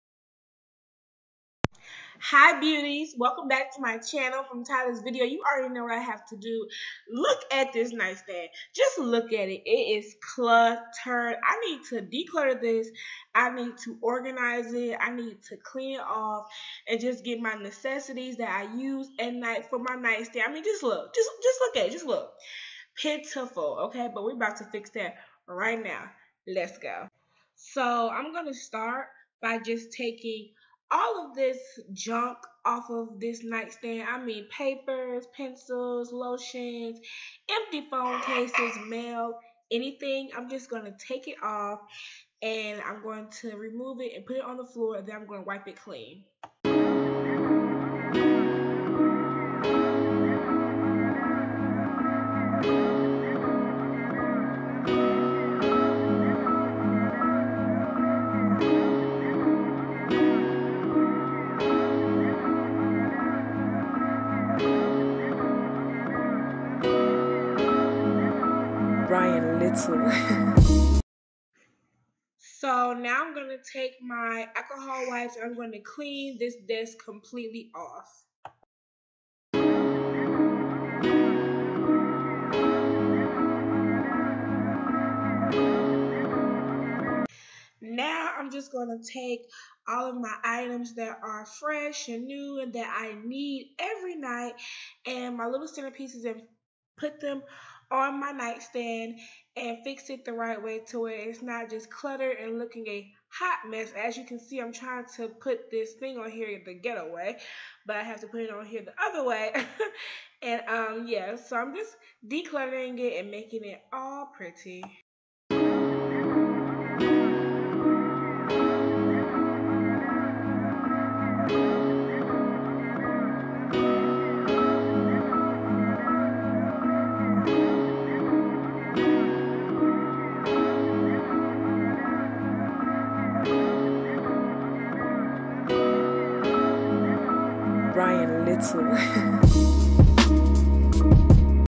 CLEAN WITH ME | DECLUTTER + ORGANIZATION 2017 | NIGHT TABLE EDITION
HEY BEAUTIES THIS IS HOW I DECLUTTER AN CLEAN MY NIGHT STAND AS YOU CAN SEE IT WAS MESSY IF YOU LIKE THESE TYPES OF VIDEOS GIVE ME A THUMBS UP AN SUBSCRIBE SO I CAN KEEP THEM COMING YOUR WAY MUCH LOVE STAY HUMBLE AN ILL SEE YOU IN MY NEXT VIDEO ❣